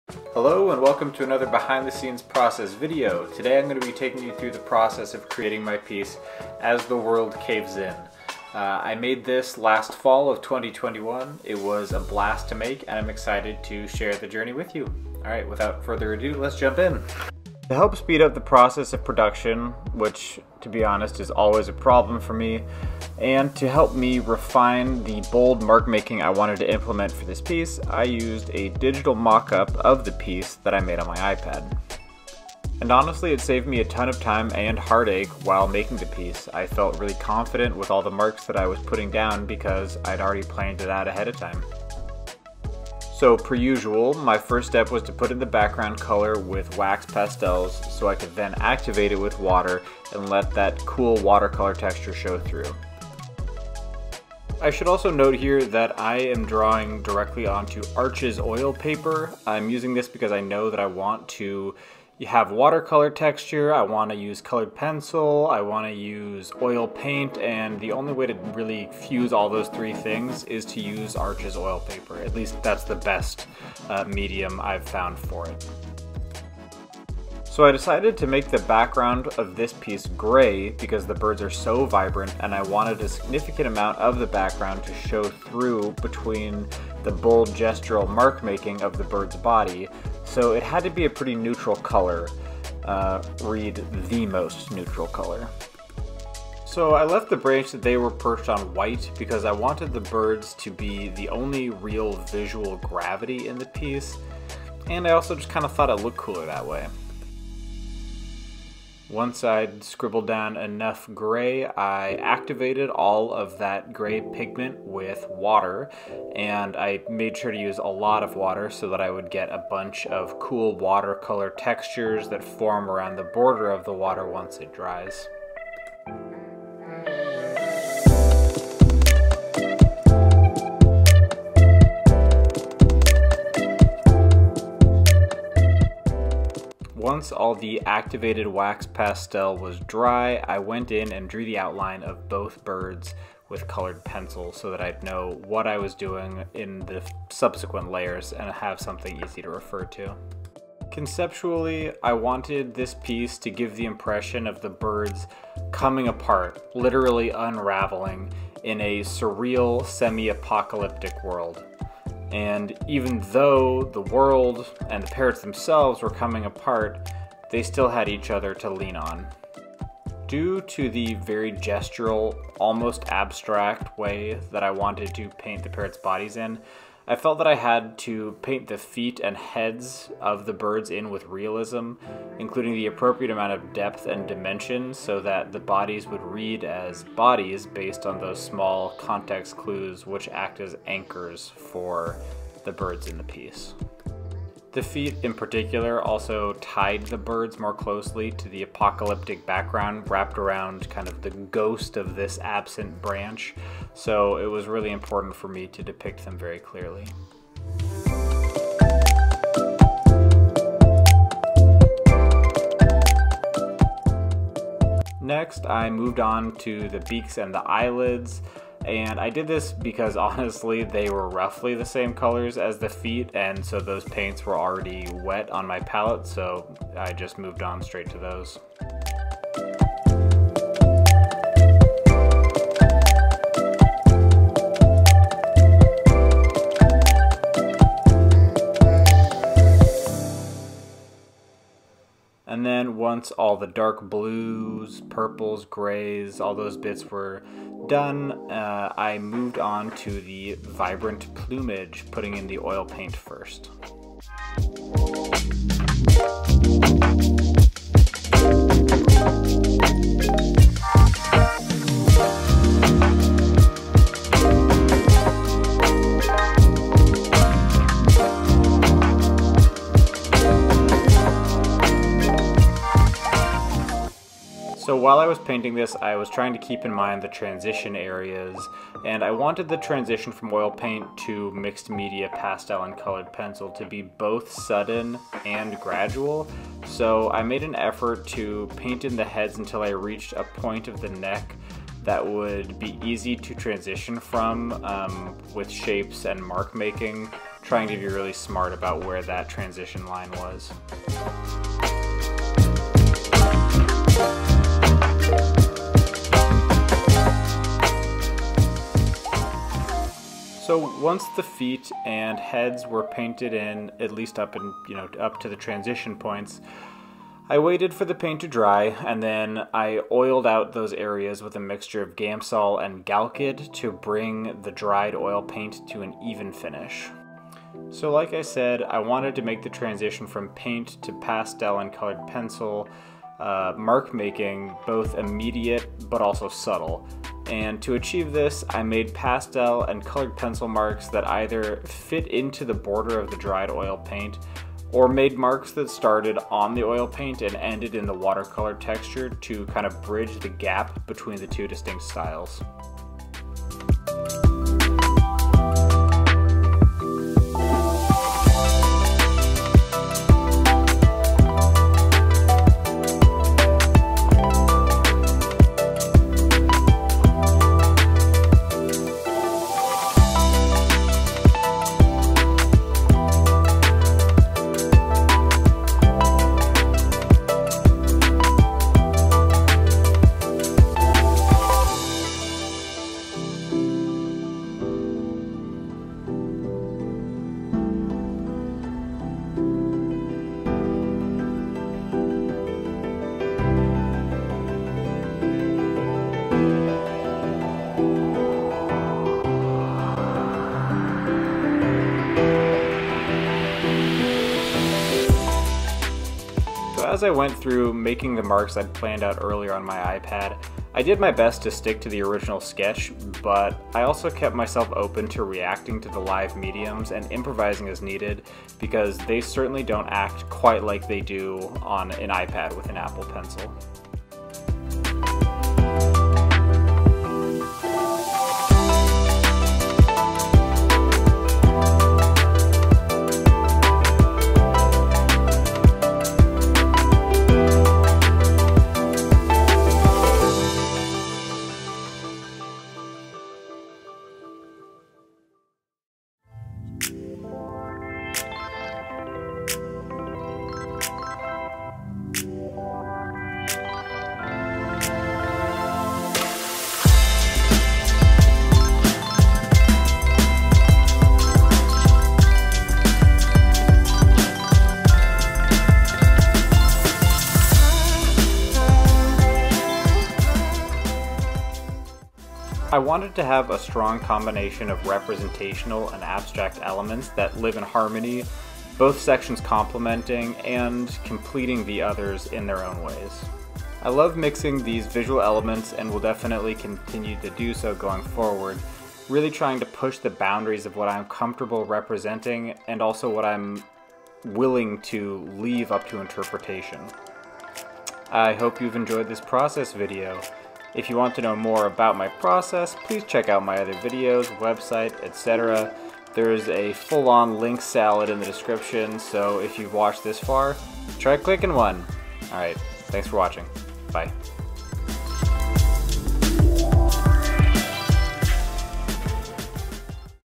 Sun conure painting - Behind the scenes
This video details the process of creating my artwork "Wander".

Timestamps:
0:00 Intro
0:17 Materials and set-up
0:43 Wax pastels
2:16 Oil paints
5:10 Mixed media
8:28 Outro

Materials:
Caran d'Ache Neocolor II water soluble wax pastels
Prismacolor colored pencils
Gamblin oil paints
Sennelier soft pastels
Arches oil paper